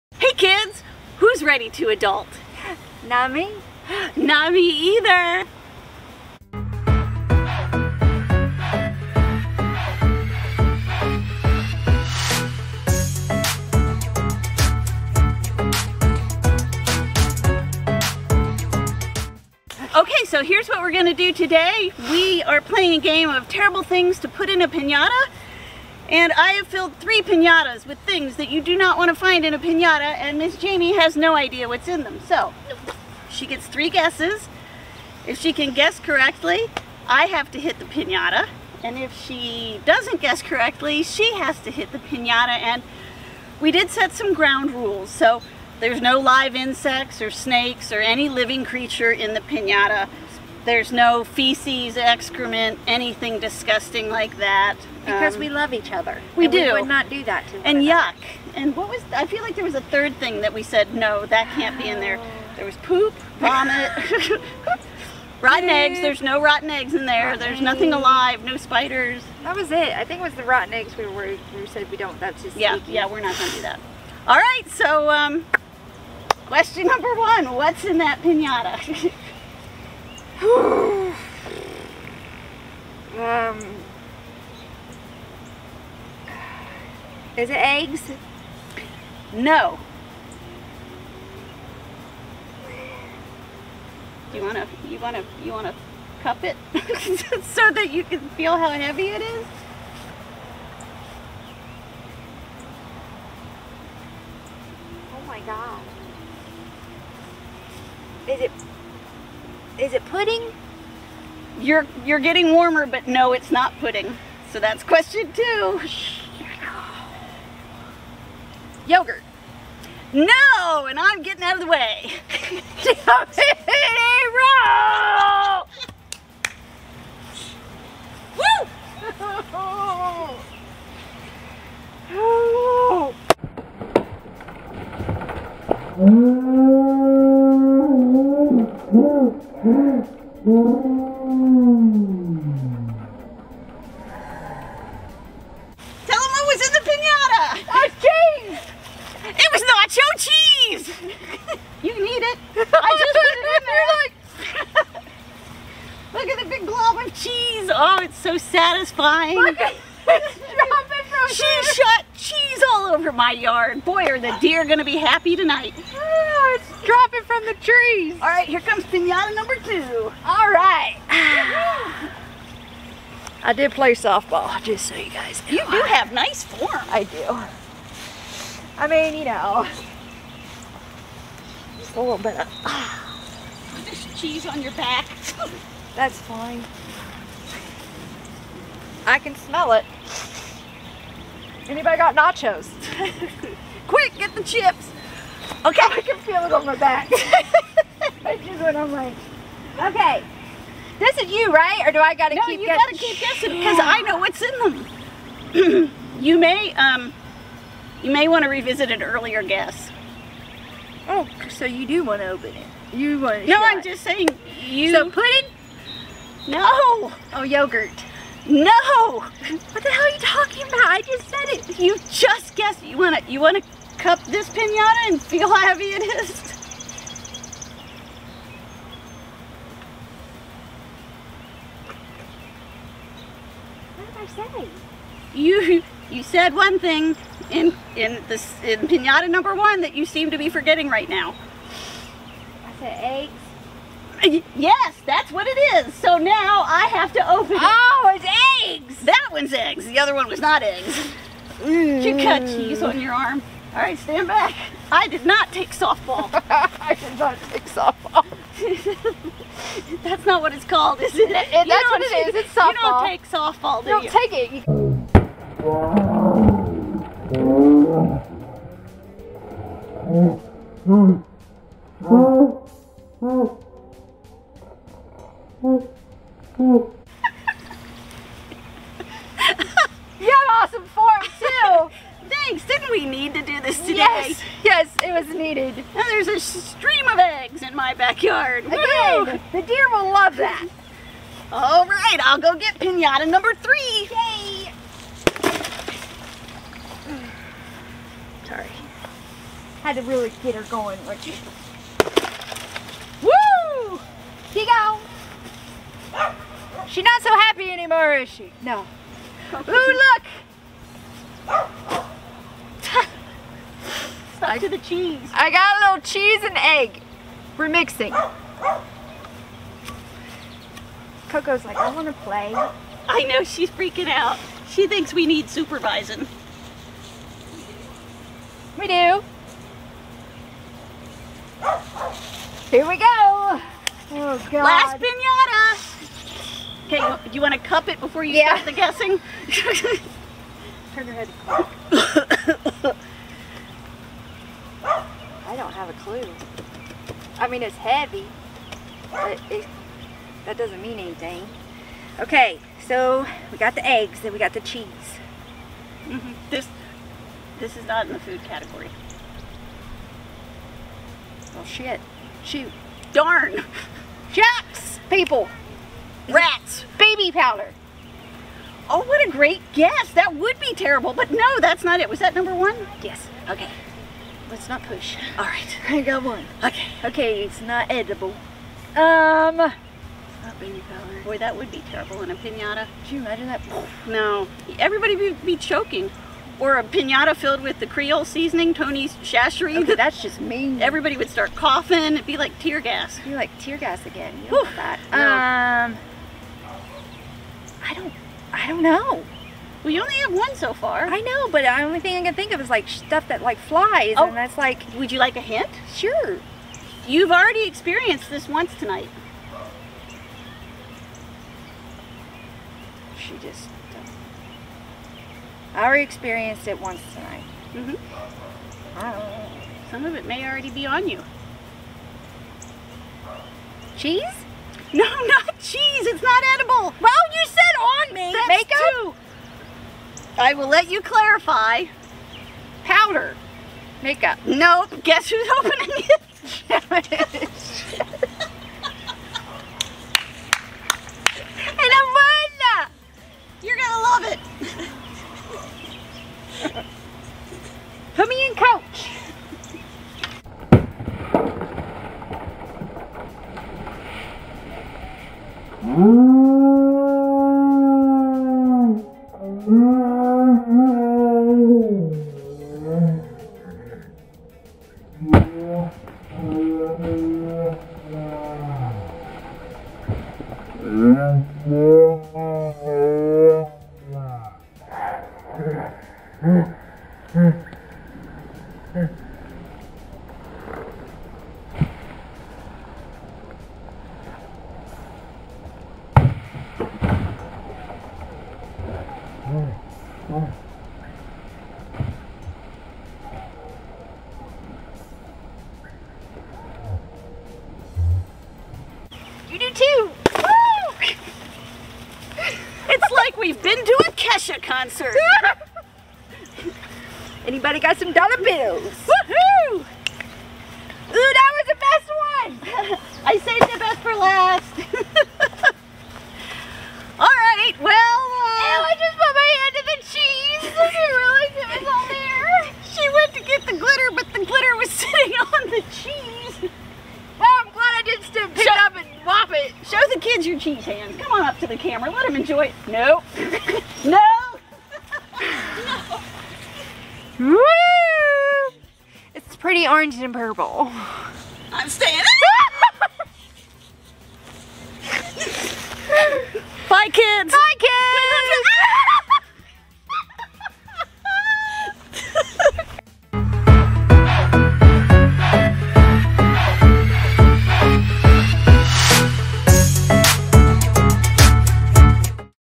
Ew! Today we're putting terrible things in a piñata!
We decided to play a game based on terrible things to put in a pinata. If Miss Jamie can guess what's in the pinata within 3 guesses, Miss April has to open it. If she can't, she has to open it! You'll never guess what we're covered in by the end of the video.